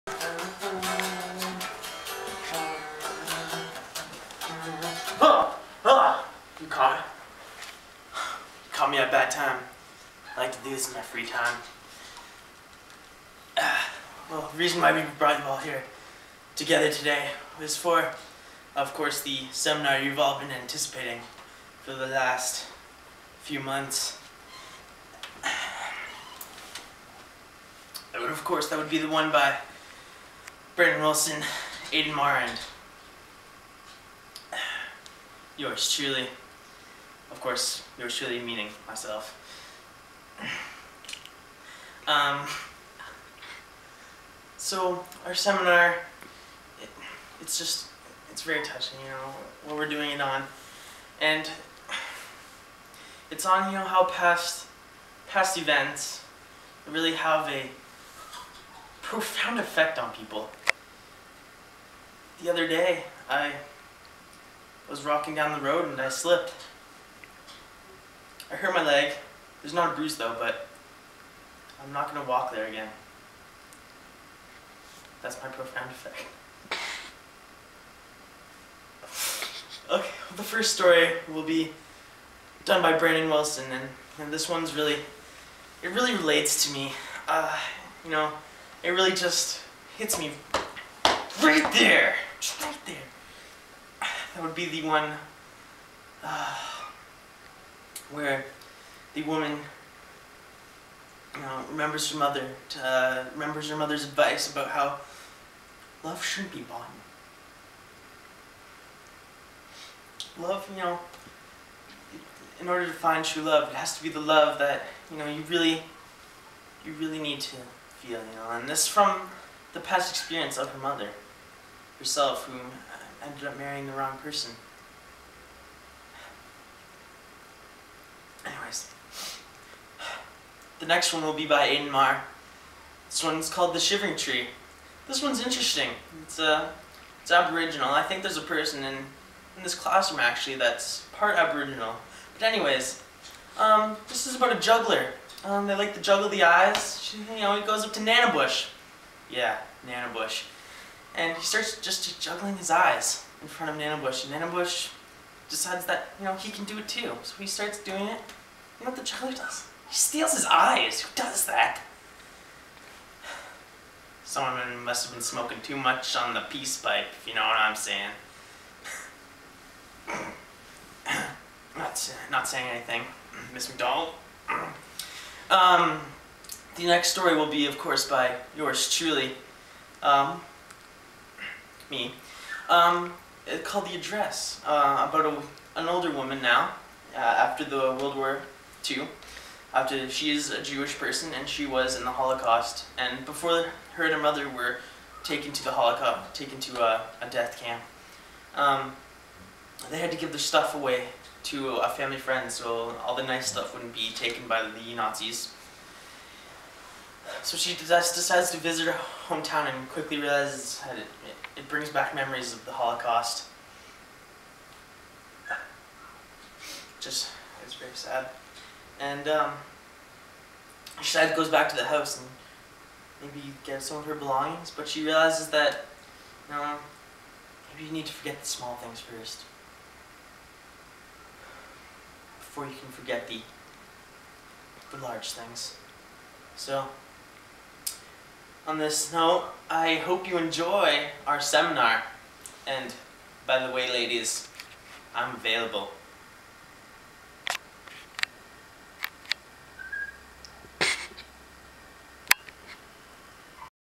Short Story Seminar Intro
This is my intro for my groups seminar for my grade 11 english class